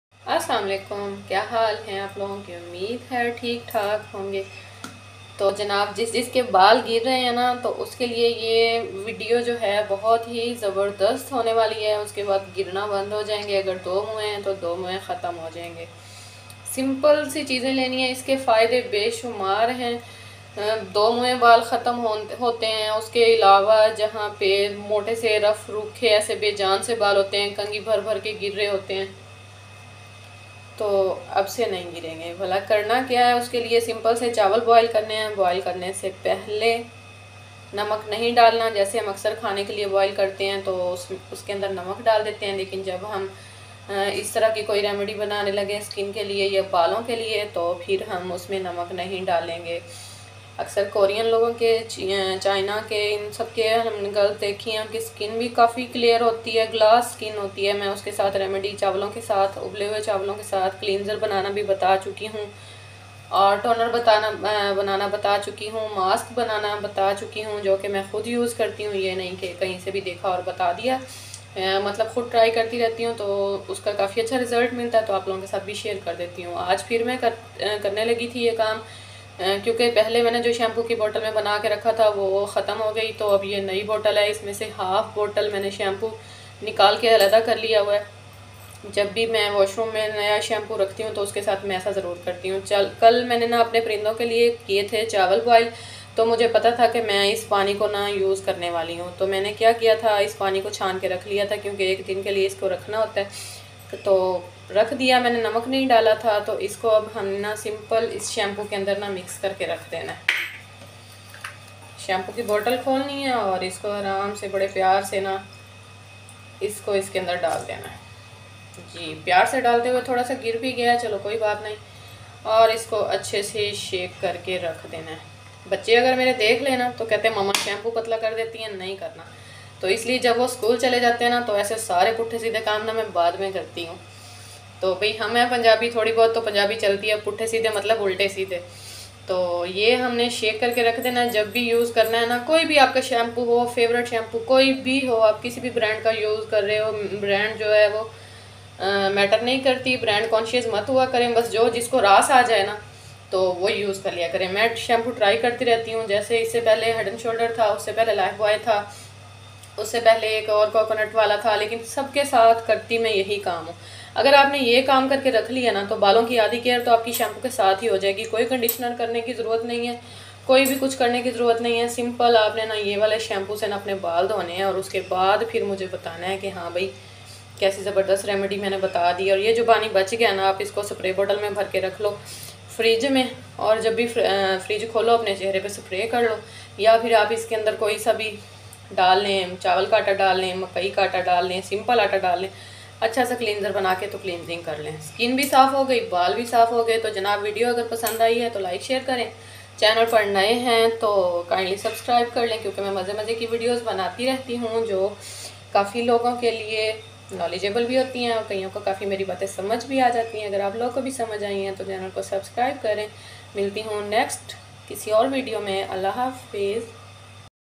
Hair fall solution| No Dandruff |you must try this remedy
diy remedy for long and shiny hairs , stopped hair fall ,no dandruff itchy scalp,work for split ends
toptrending video
gardening and cooking with saima